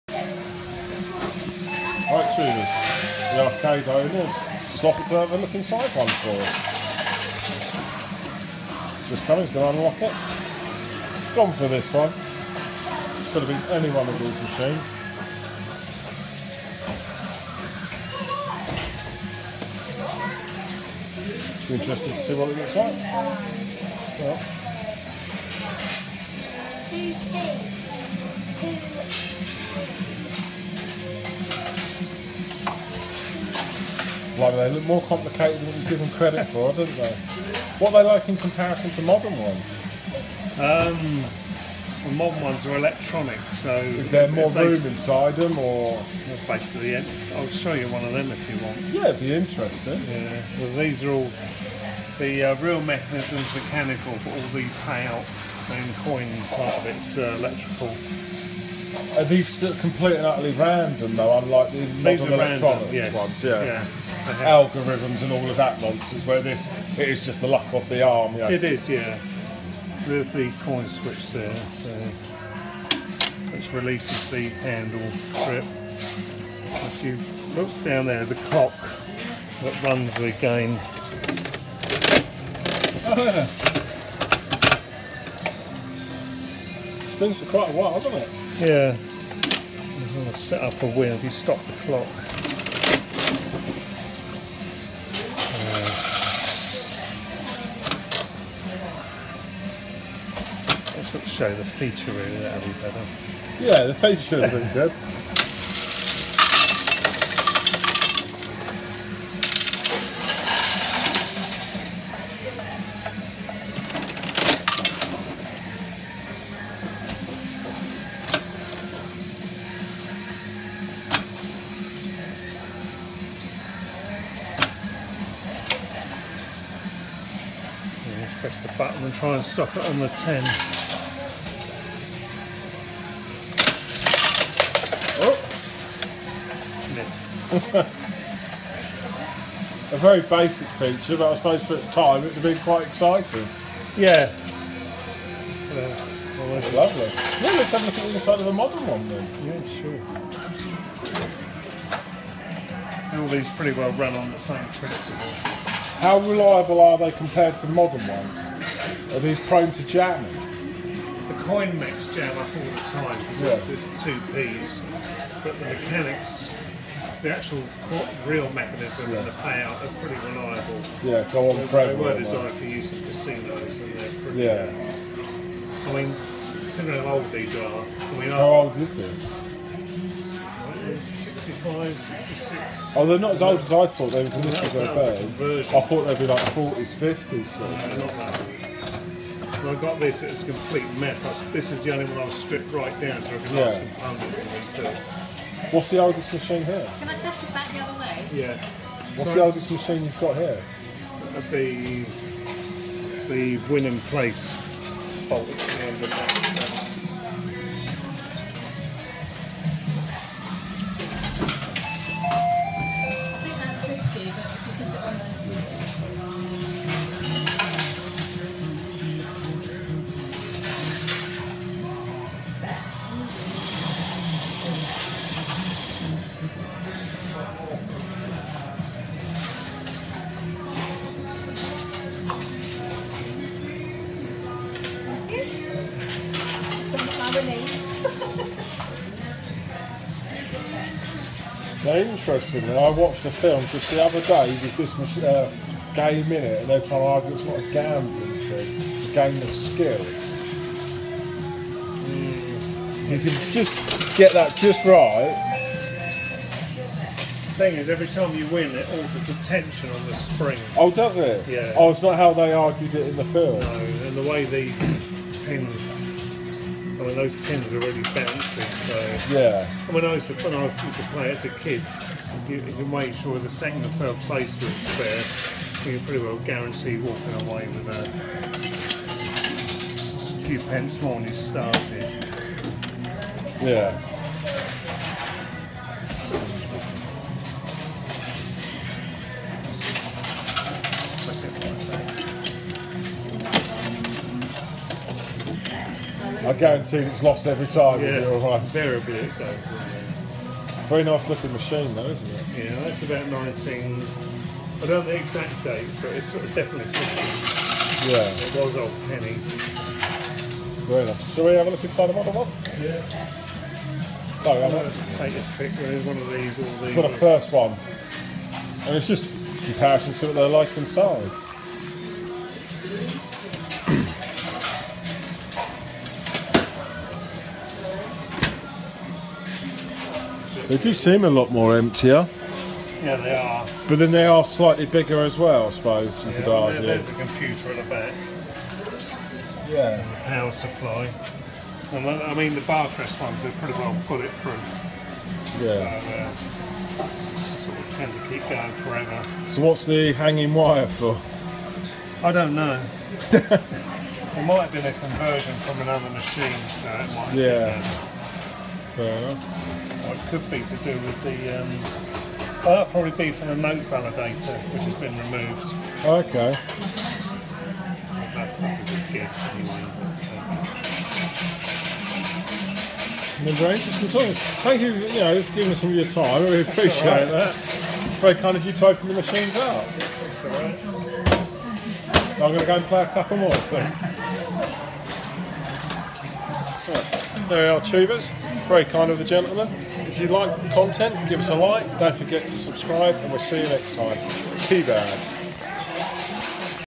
Inside a retro fruit machine Shanklin Isle of Wight
Hey Tubers

while in the retro arcade, I always ask if I can film, because its the right thing to do, the owner was more then happy to let me so much in fact he wanted to help out by showing of the insides of the machines,

fantastic